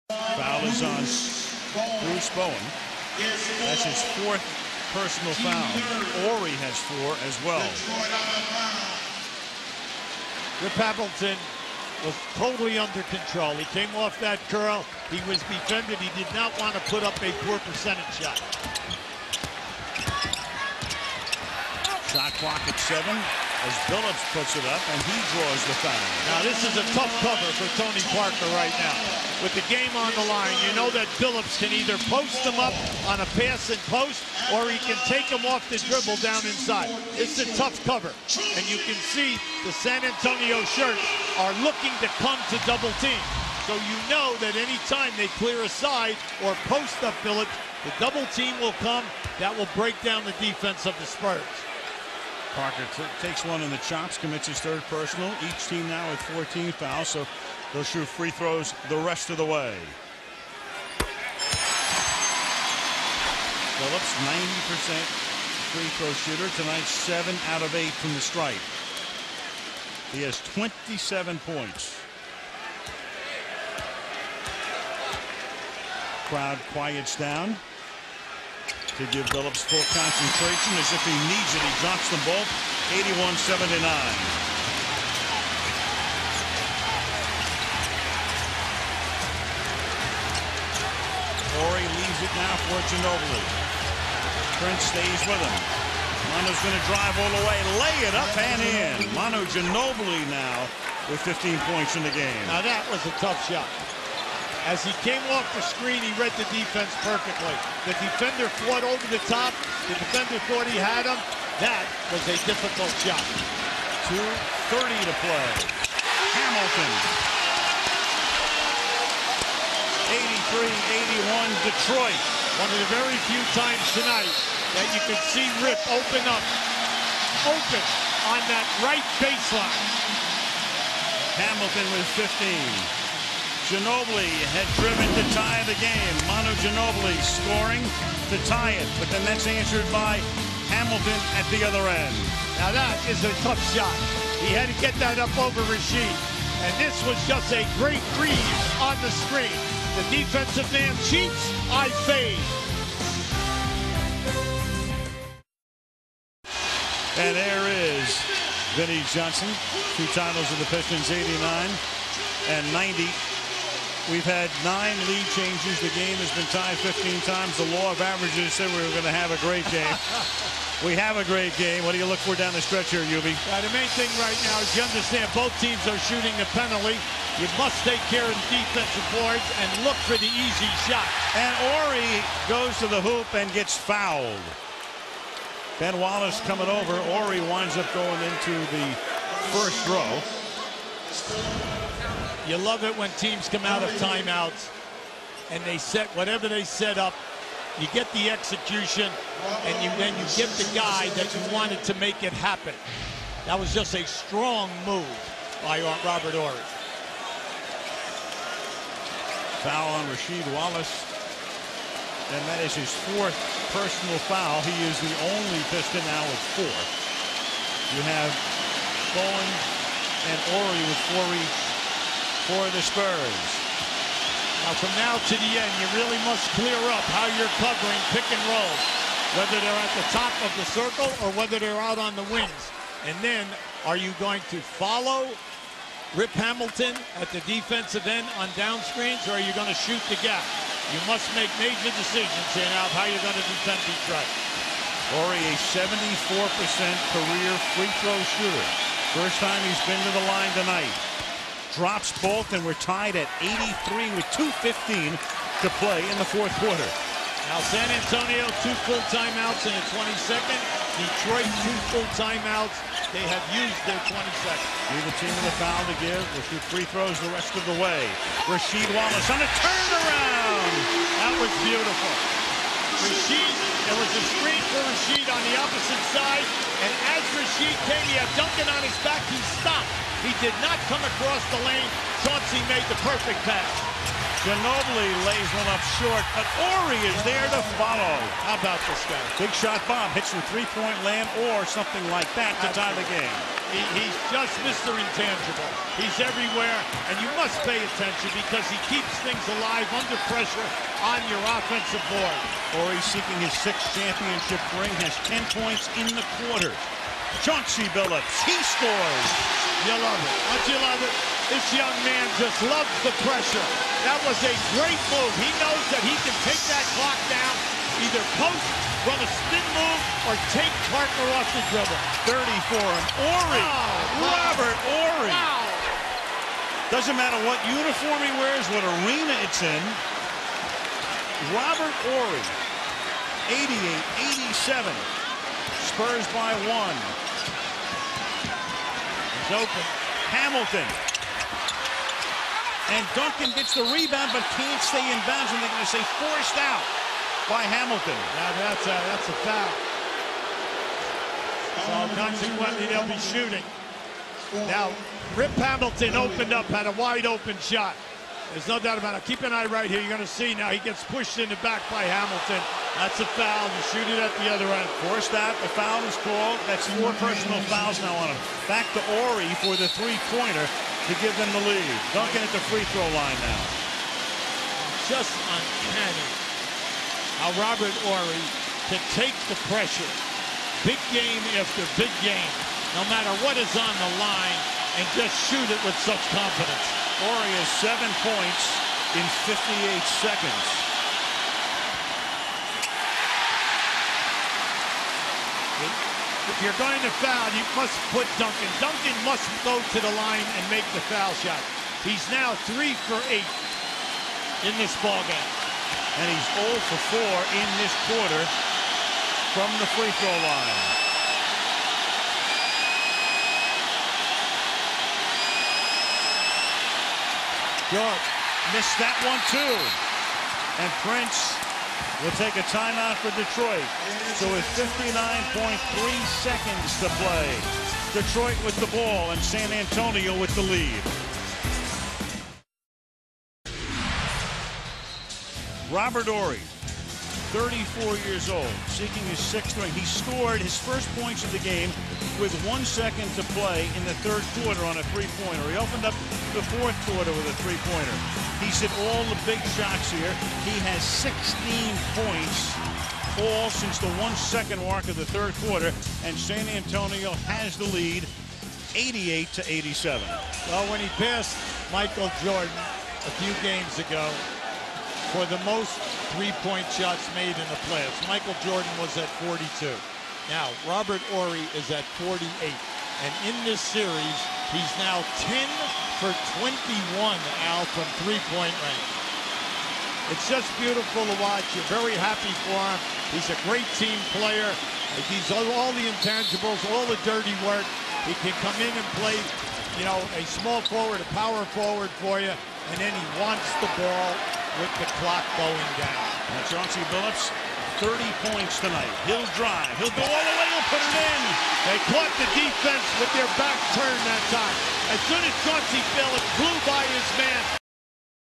2005 NBA Finals Gm 5 - Robert Horry Heroics Part 1
Horry carries the Spurs to a crucial game 5 victory on the road against the Pistons in the 2005 NBA Finals